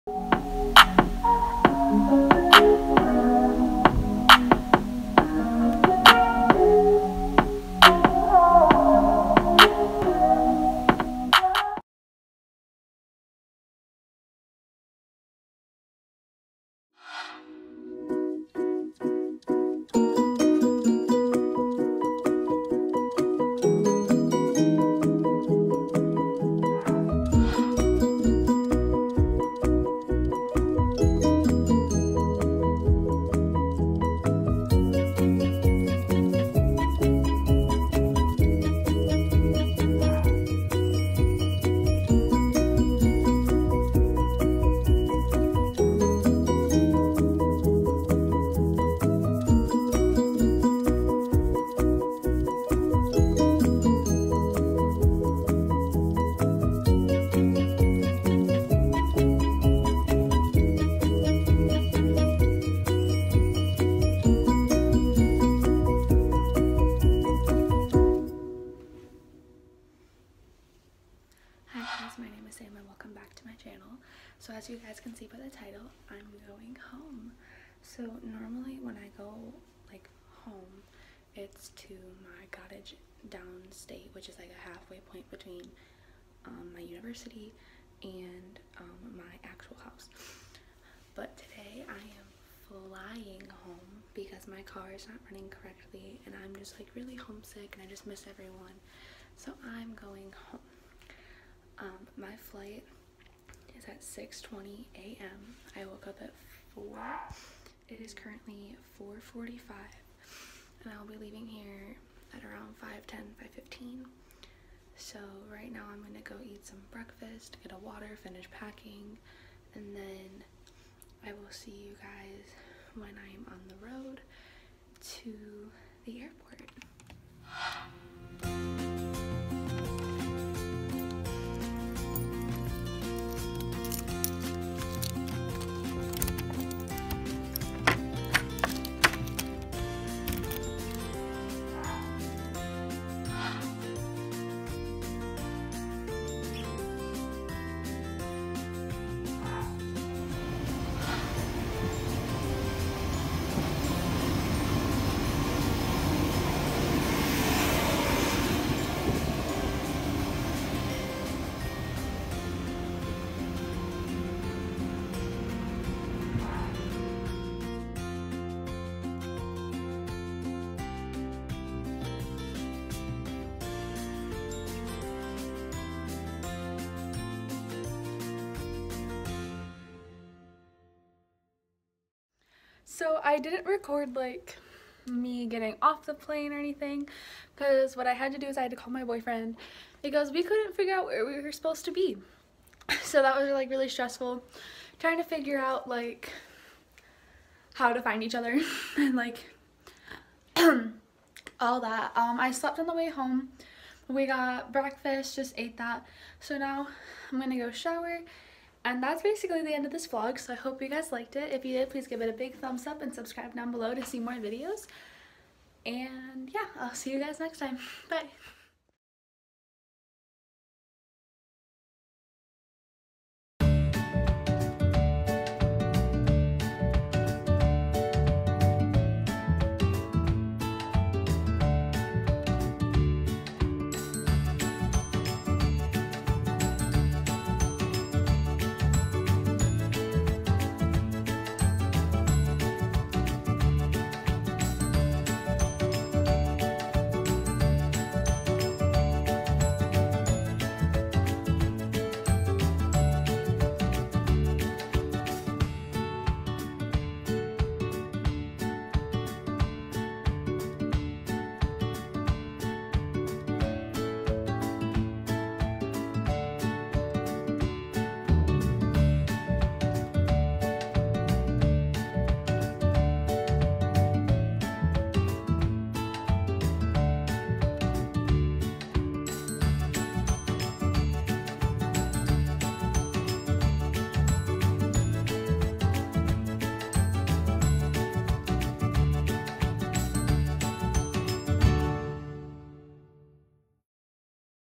Travel home with me vlog
I went home!

With finals and college being stressful I was not able to get this video up as soon as I wanted it to.

I hope you guys enjoy this video! And if you ever are home sick, go home for a weekend or week, it’s worth it.